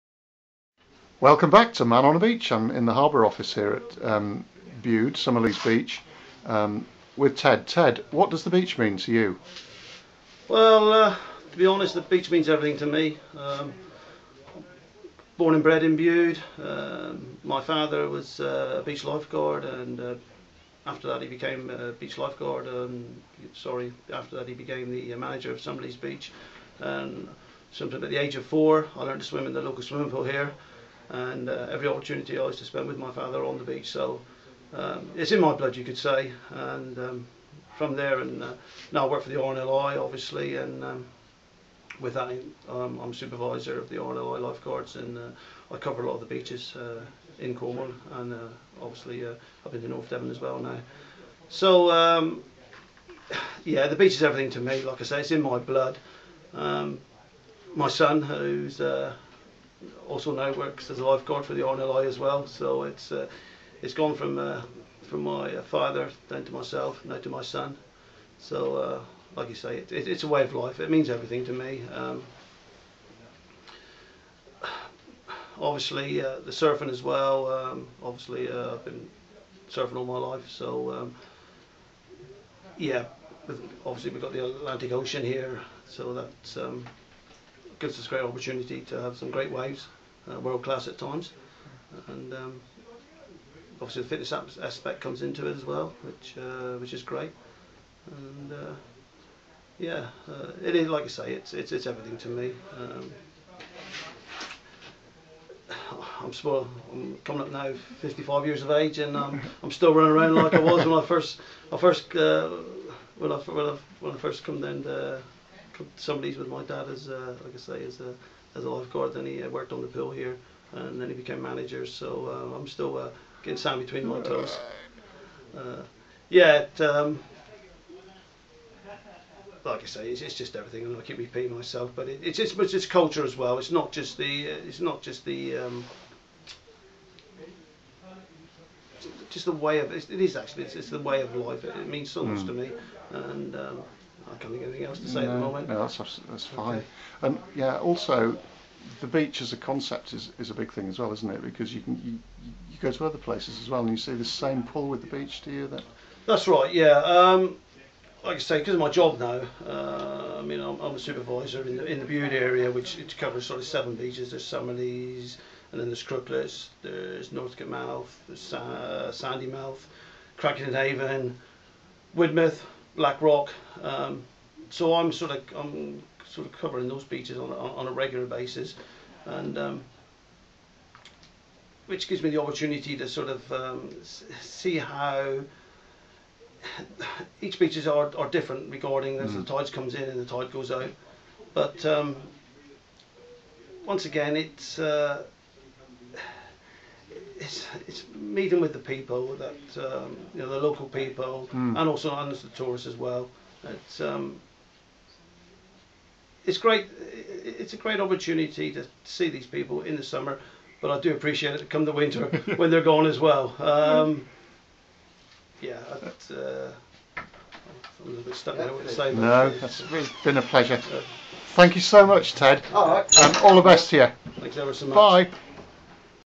Summerleaze Interview 2
With Ted on Summerleaze beach, Bude in Cornwall, from manonabeach.